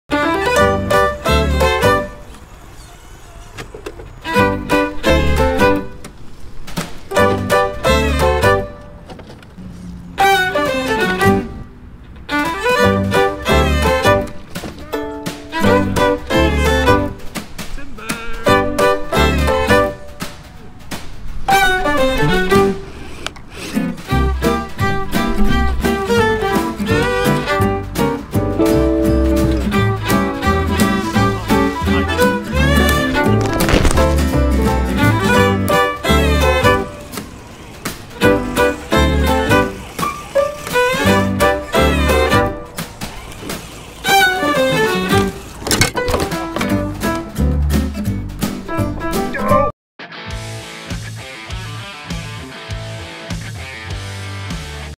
Try not to laugh
In this weeks epic RC Adventure crawling fails, The Axial SCX10 3 and the Traxxas TRX4 Tumble and roll in these school boy error filled clips.